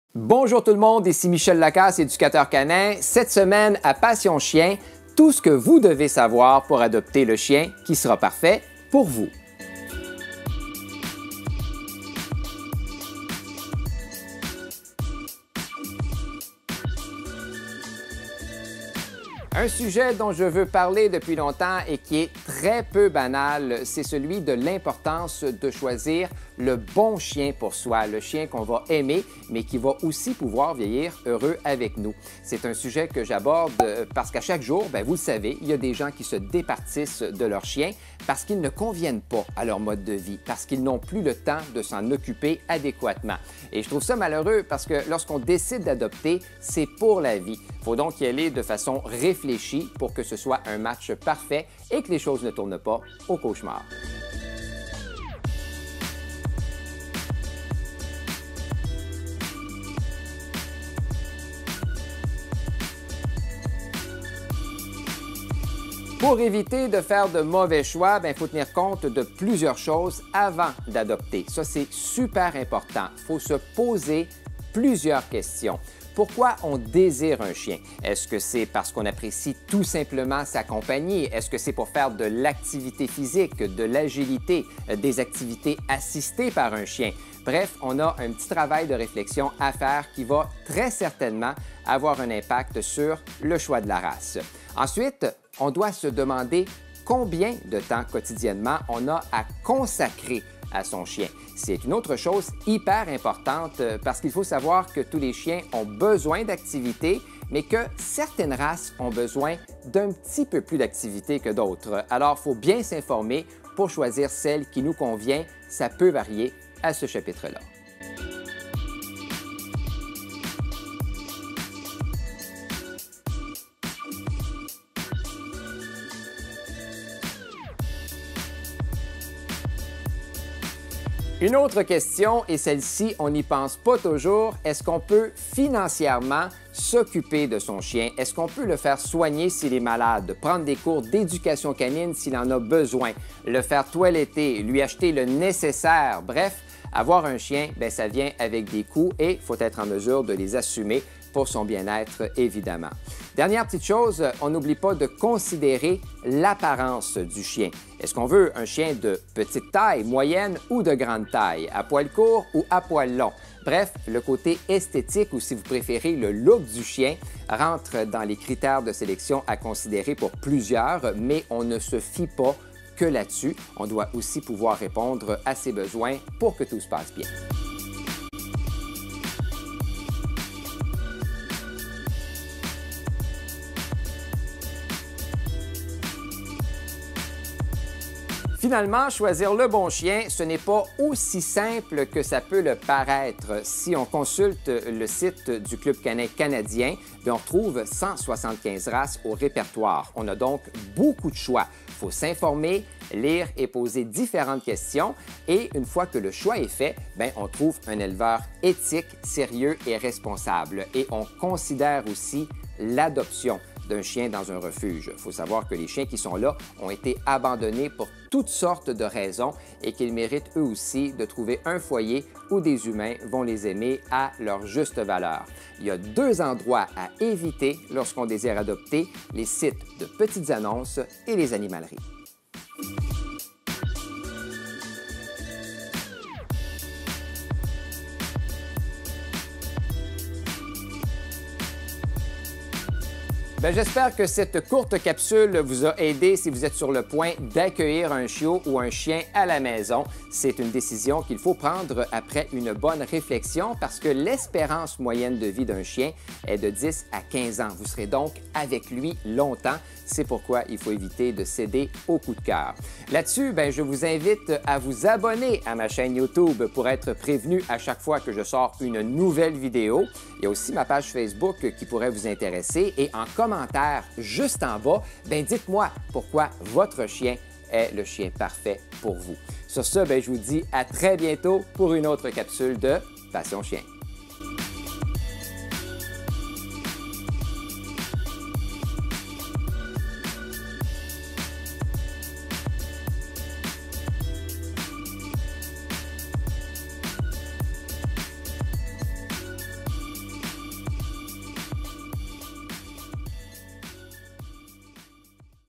⚠️ Comment CHOISIR la BONNE RACE de CHIEN 🐕
Choisir la race de chien qui nous convient demande une réflexion et un bon travail de recherche. Il faut tenir compte de plusieurs critères et se poser nombre de questions,. L'éducateur canin Michel Lacasse fait le point sur le sujet dans cette capsule de Passion chien :)

Pour suivre Michel Lacasse sur...